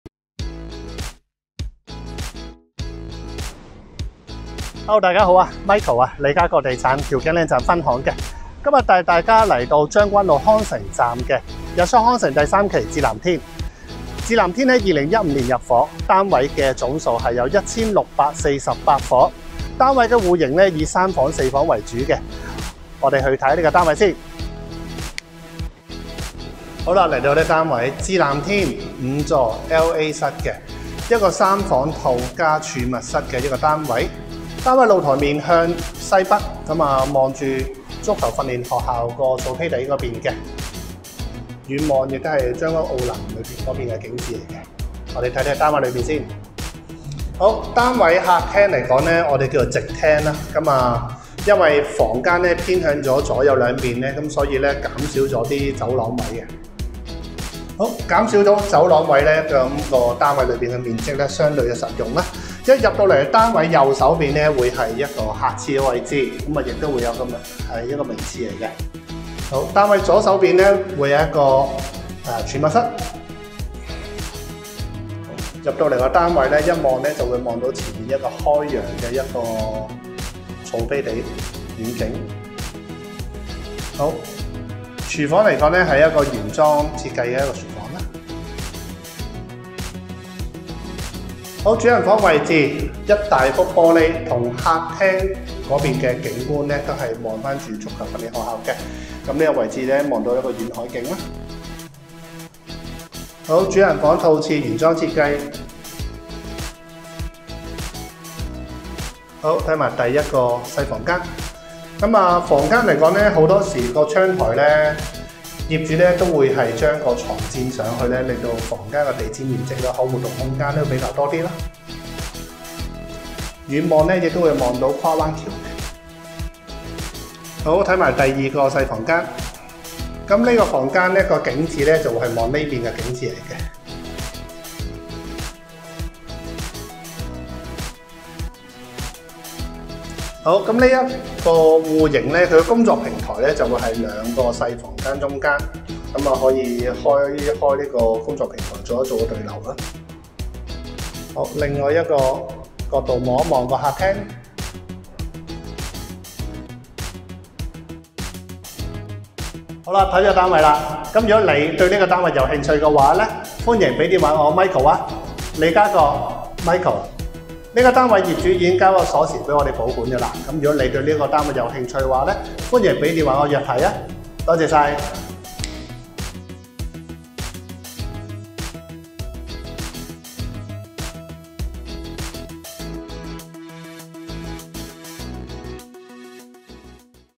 【KOL睇真盤】 Michael Wong 筍盤推介：日出康城 緻藍天 晶巒 (5座)中層 LA室
KOL睇真盤 – Michael Wong 筍盤推介：
將軍澳  日出康城
物業地址：日出康城 緻藍天 晶巒 (5座)中層 LA室（物業編號: BY25666019）
實用面積： 769呎
間隔 ：三房套+儲物室
賣點 ： 調景嶺站. 站頂屋苑